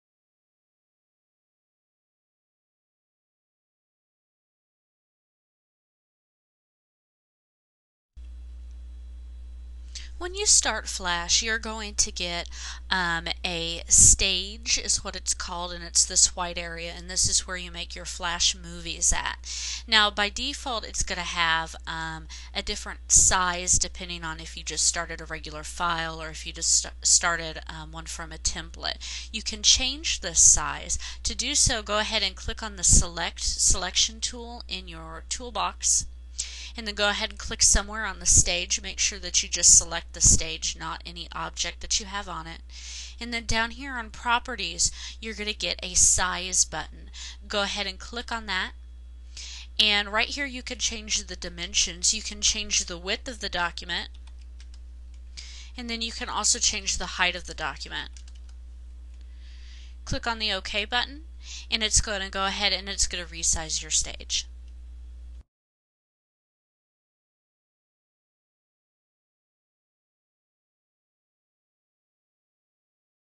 Resize the Stage in Flash CS3
See a higher quality video on TechTutor.TV! The standard stage that appears when you start a new project in Adobe Flash CS3, can be resized easily. In this video tutorial, you will learn how to resize the stage in Flash CS3. See higher quality tutorials all for FREE